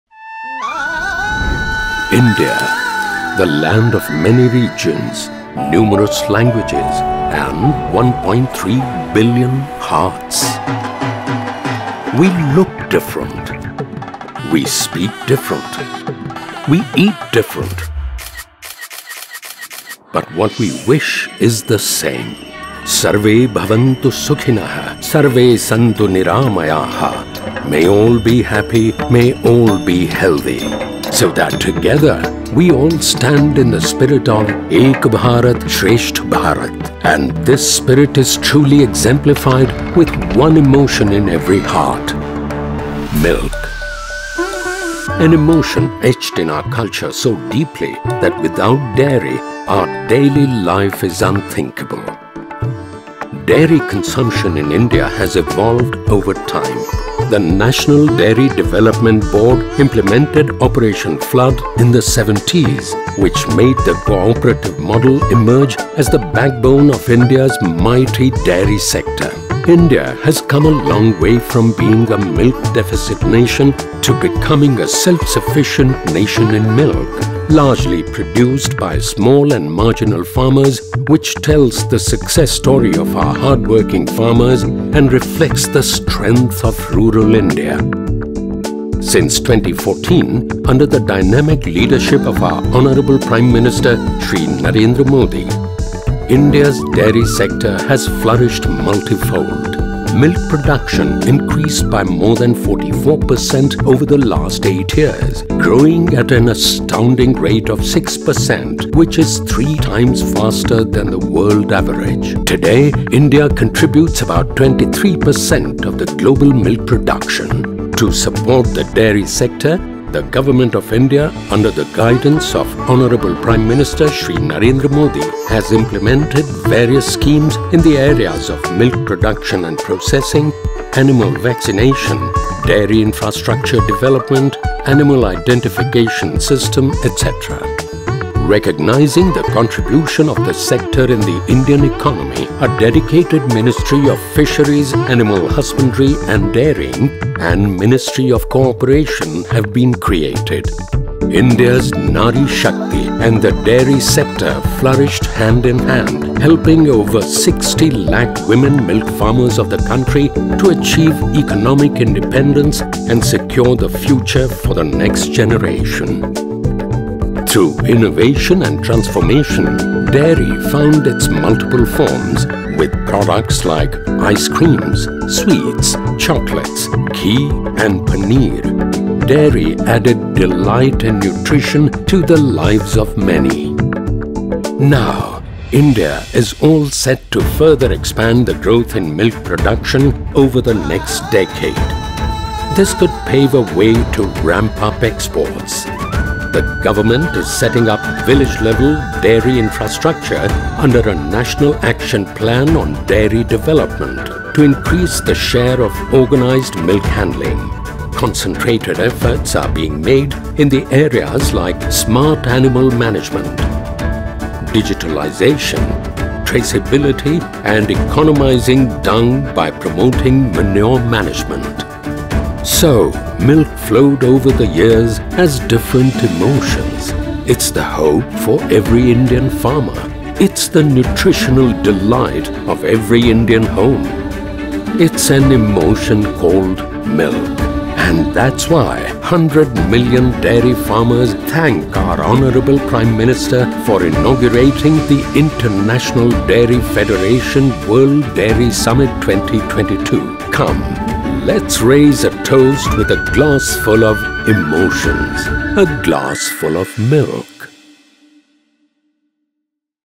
International Dairy Federation World Dairy Summit 2022: Creating a vibrant dairy sector
Prime Minister Narendra Modi will inaugurated International Dairy Federation World Dairy Summit (IDF WDS) 2022, at India Expo Centre & Mart, Greater Noida, on 12th September 2022.

The four day long IDF WDS 2022 is a congregation of global and Indian dairy stakeholders including industry leaders, experts, farmers and policy planners centring around the theme of ‘Dairy for Nutrition and Livelihood’. Around 1500 participants from 50 countries participated in IDF WDS 2022. The last such Summit was held in India about half a century ago in 1974.

The Indian dairy industry is unique in the sense that it is based on a cooperative model that empowers small and marginal dairy farmers, especially women. Driven by the vision of the Prime Minister, the government has taken multiple steps for the betterment of the dairy sector resulting in an increase of milk production by more than 44% in the last eight years. The success story of the Indian dairy industry, accounting for about 23% of global milk, producing around 210 million tonnes annually, and empowering more than 8 crore dairy farmers, was showcased at the IDF WDS 2022. The summit also helped Indian dairy farmers to gain exposure about the global best practices.